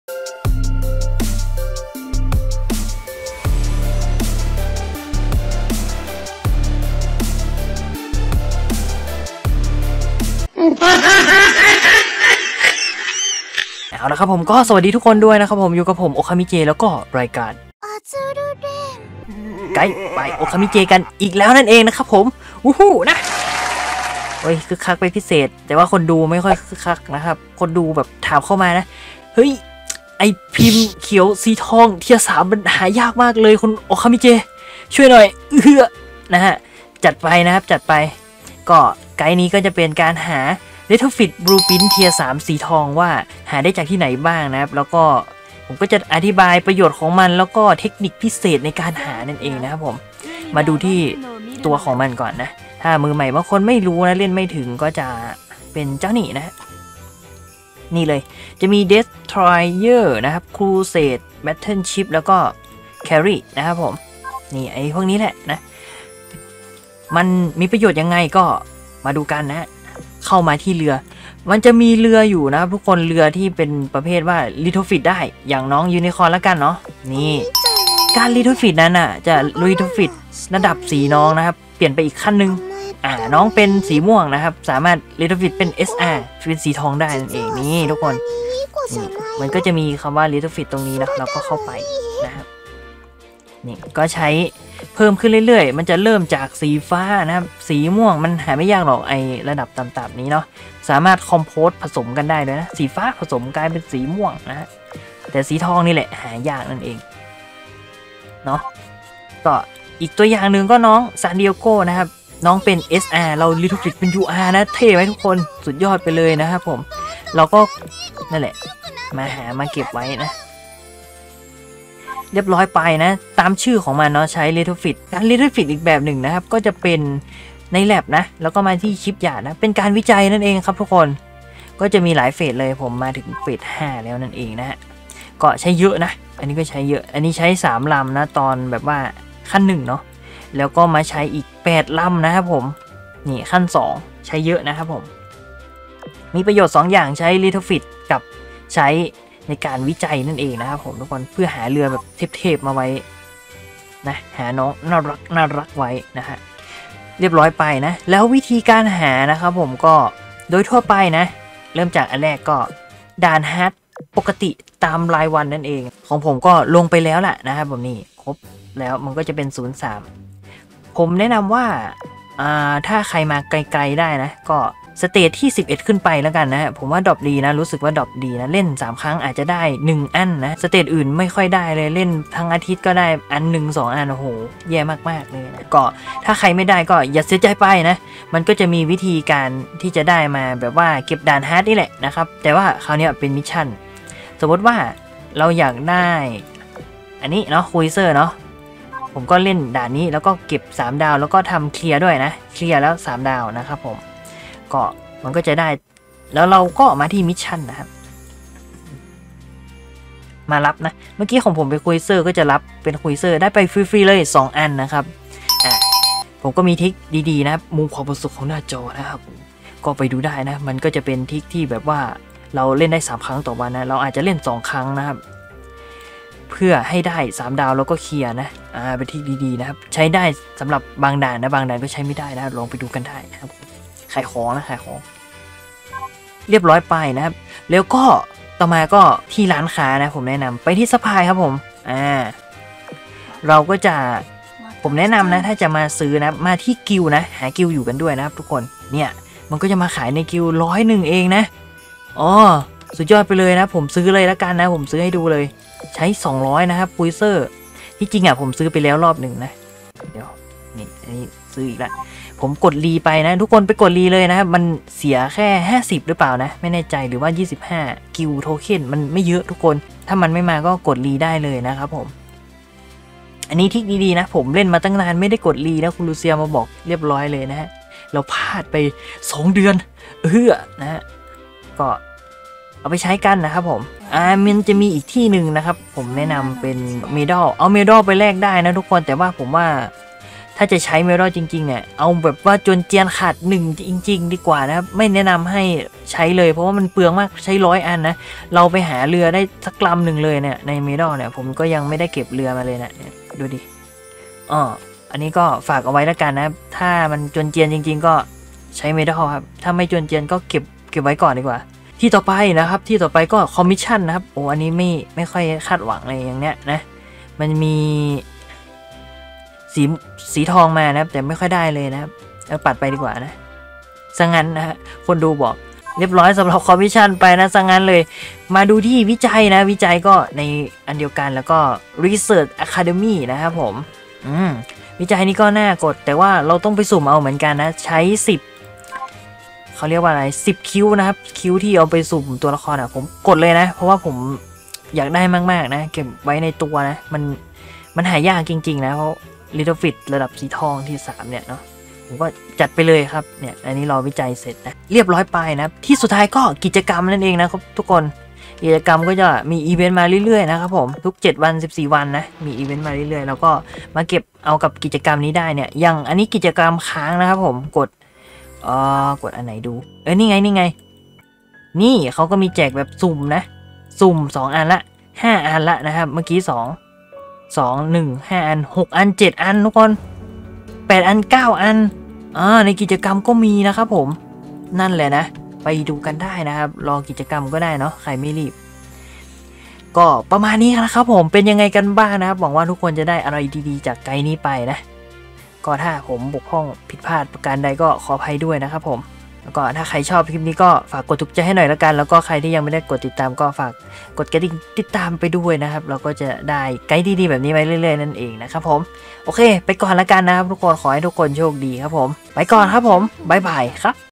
Ep.9 แนะนำมือใหม่ หา Retrofit Blueprint T3 ได้จากที่ไหนบ้าง?
0:38 - เริ่มไกด์
0:45 - ประโยชน์ของ Retrofit Blueprint T3
2:43 - หาจากด่าน Hard
3:56 - หาจากในร้านค้ากิลด์
5:12 - หาจากส่งไปทำ Commission
5:25 - หาจากการวิจัย
5:55 - หาจากกิจกรรม Event ต่างๆ

ดูสิทธิพิเศษสำหรับการเป็นสมาชิก

ถ้าชอบคลิปนี้อย่าลืมกดติดตาม กดไลค์ กดแชร์ เพื่อรับชมคลิปใหม่ๆ ด้วยนะครับขอบคุณครับ